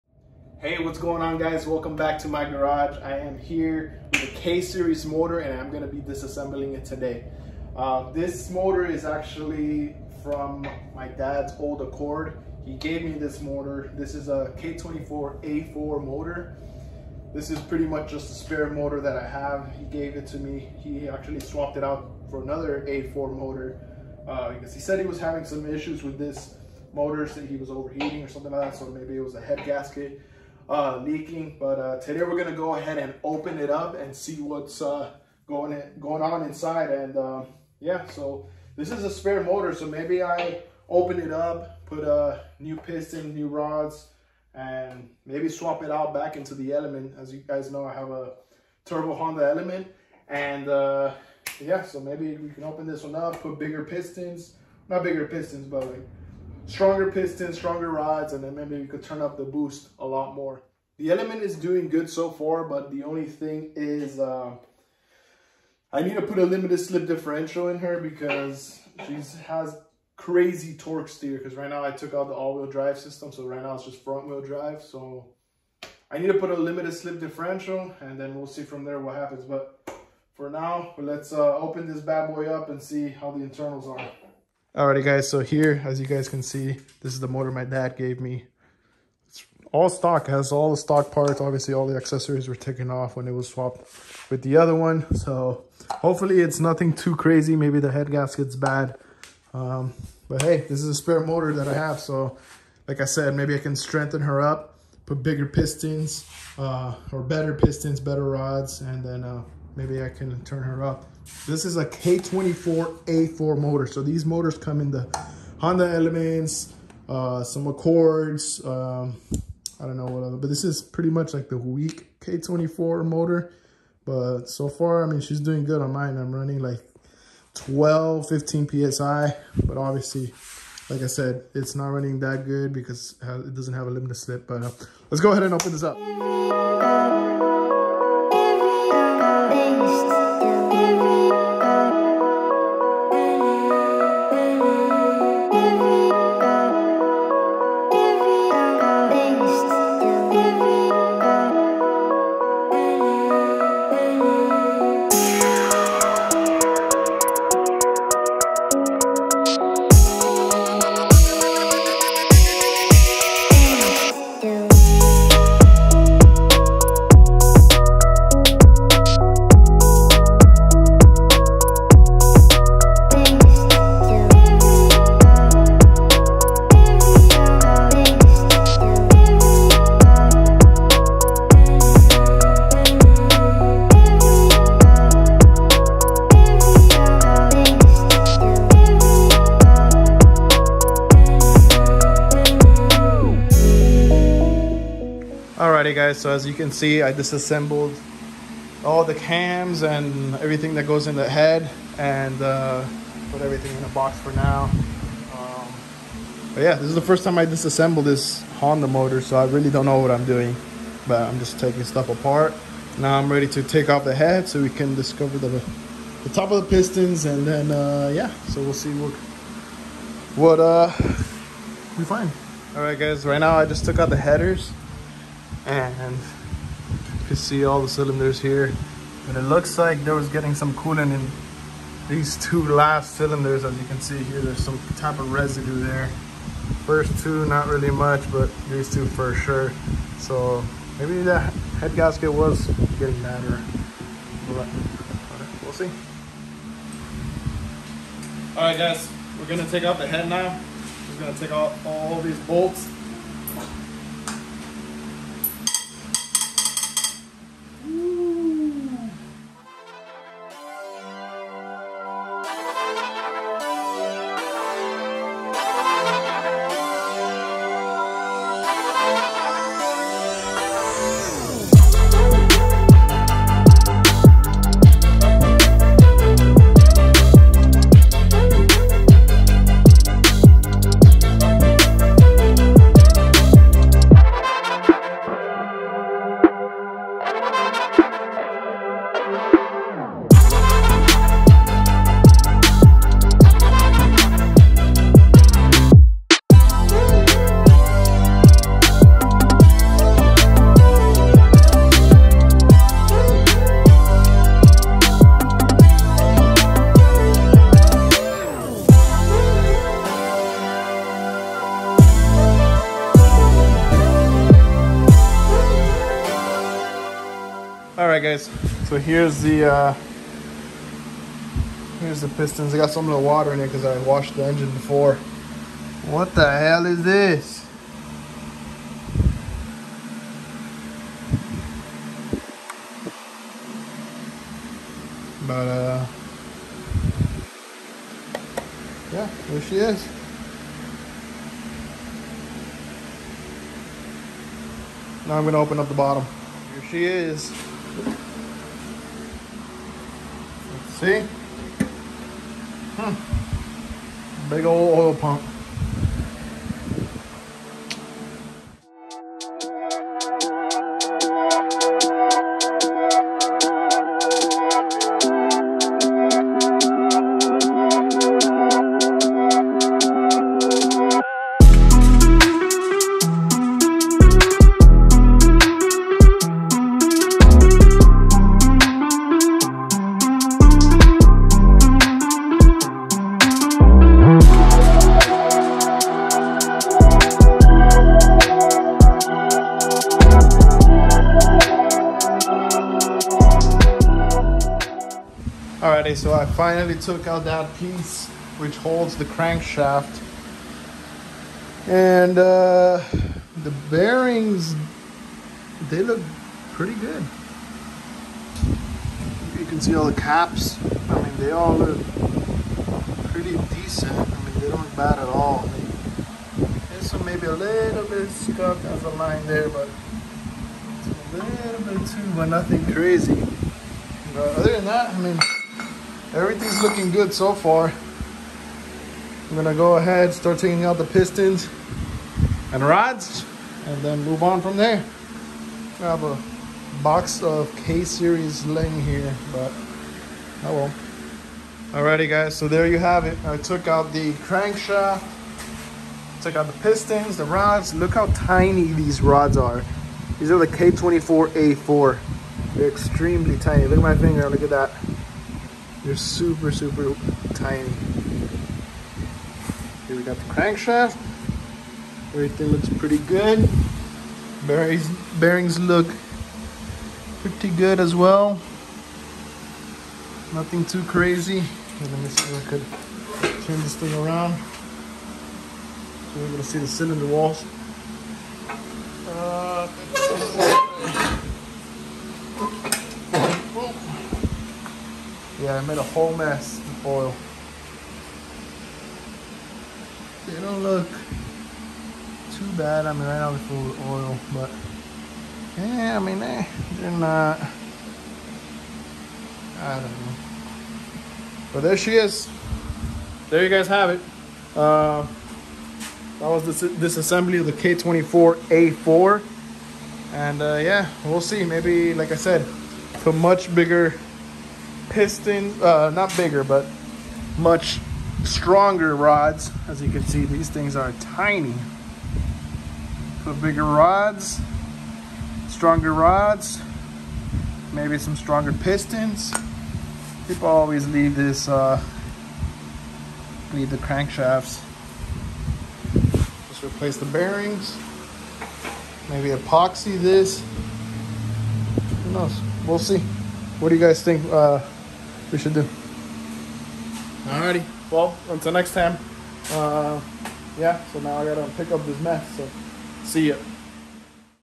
Honda K24 Engine Teardown ⚙️
This is the first time I take apart a Honda engine! I honestly do not know what I am doing, but that is how you learn. I will be disassembling this K24A4 off of a Honda Accord. Anyone have suggestions on how to make this engine bullet proof? I will probably end up beefing up this K24 and swapping it over to my Turbo Honda Element for more power! Let me know if you would like to see more of this type of content!